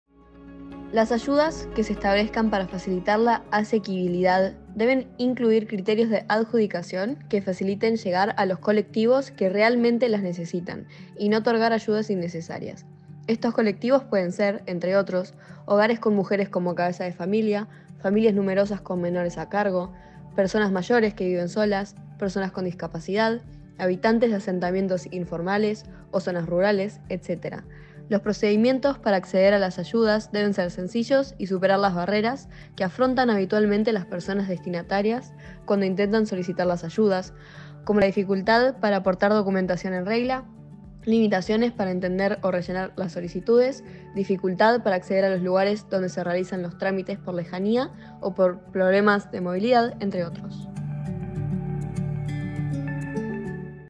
Derecho Humano al Agua - Asequibilidad- Fácil acceso a ayudas
Para cumplir este criterio, la municipalidad debe asegurar que las ayudas puestas en marcha para permitir que el acceso al agua de consumo humano sea asequible para toda la población van dirigidas a las personas y colectivos que realmente las necesitan, evitando otorgar ayudas generalizadas innecesarias.

Tenga en cuenta que estos colectivos pueden ser, entre otros, hogares con mujeres como cabeza de familia, familias numerosas con menores a cargo, personas mayores que viven solas, personas con discapacidad, habitantes de asentamientos informales o zonas rurales, etc.

También es imprescindible que los procedimientos para acceder a las ayudas sean sencillos. Es importante evitar que algunas personas que necesiten las ayudas queden fuera por no poder aportar documentación en regla, no entender o no poder rellenar las solicitudes, no poder acceder a los lugares donde se realizan los trámites por lejanía o problemas de movilidad, o cualquier otro impedimento.